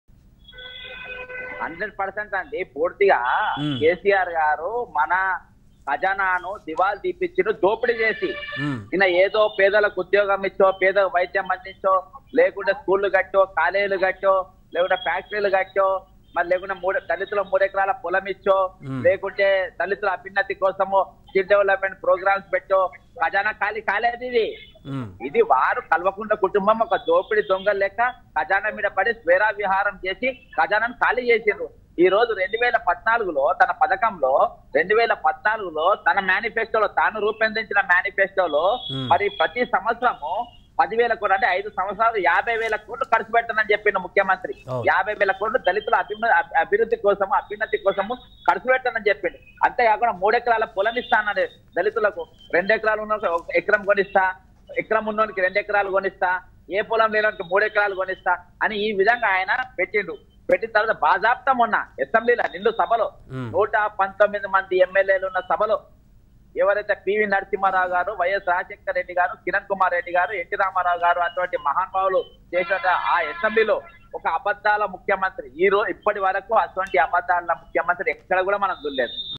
అసెంబ్లీలో ఇంత పచ్చి అబద్ధాలు ముఖ్యమంత్రి కూర్చోవడం దురదృష్టకరం | Raj news telugu
అసెంబ్లీలో ఇంత పచ్చి అబద్ధాలు ముఖ్యమంత్రి కూర్చోవడం దురదృష్టకరం | Rajnewstelugu